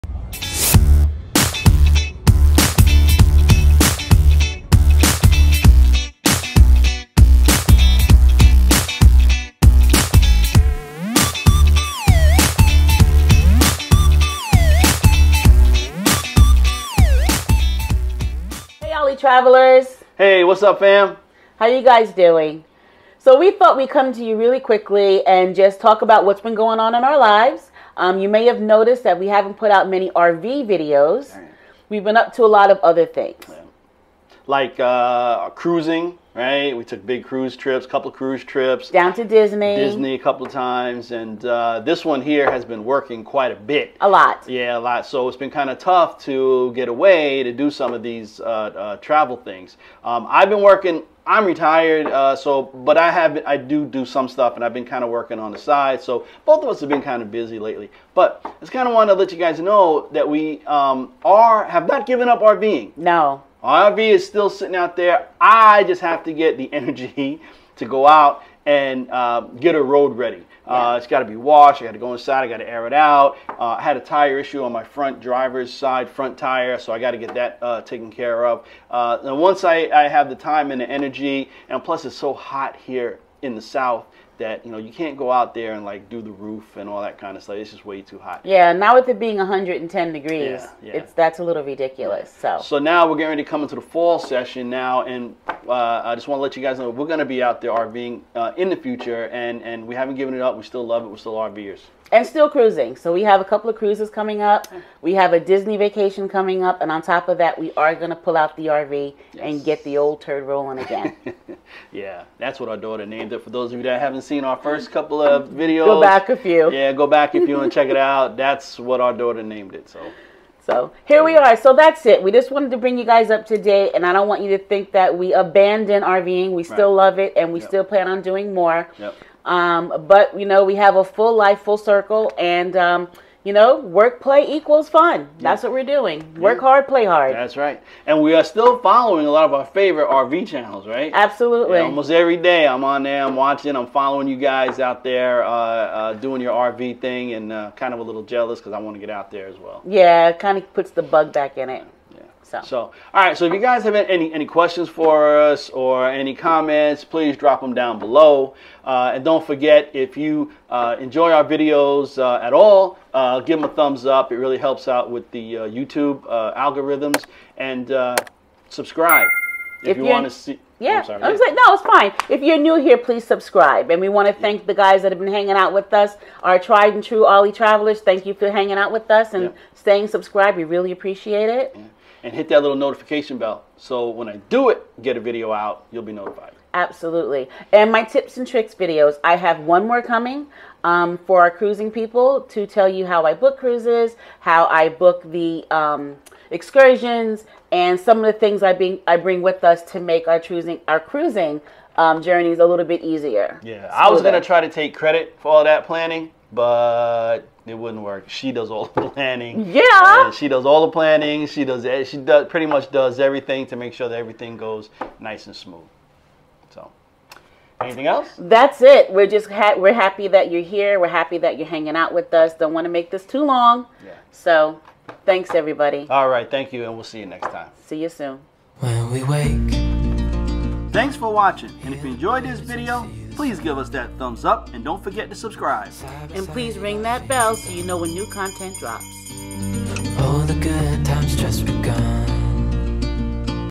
Talking RV Life
This is a short update to our RV travels and some of the other travel plans we have up coming